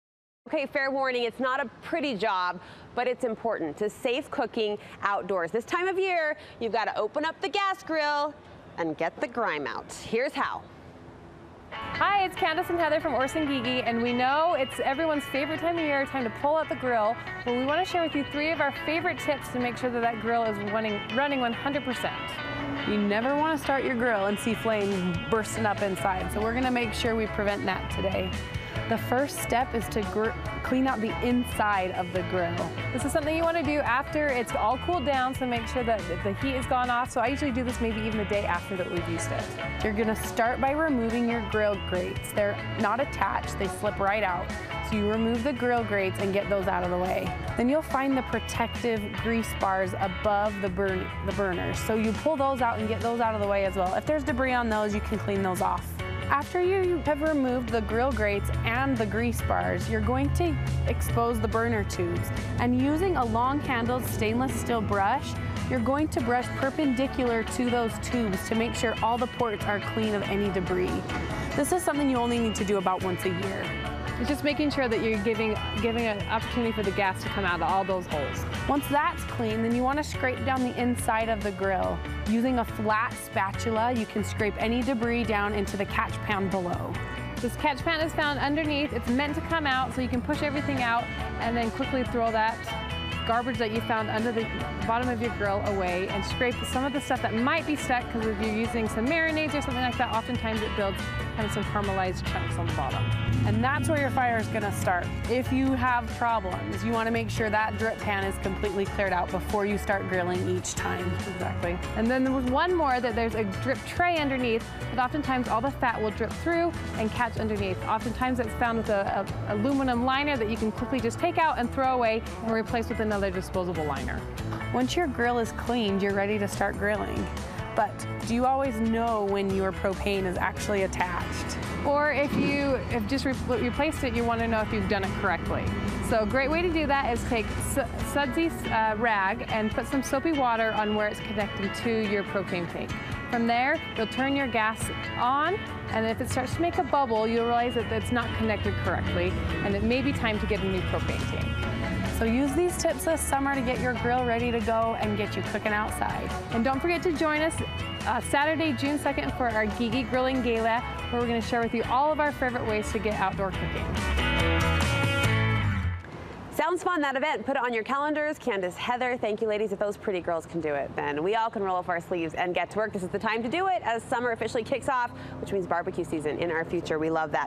Deep Clean Your Grill for Safe Summer Cooking
ORIGINALLY AIRED MAY 2018

Before you fire up the grill, you need to clean out all the grime from last summer.

Candace Heward and Heather Smith share the best way to get your grill nice and clean.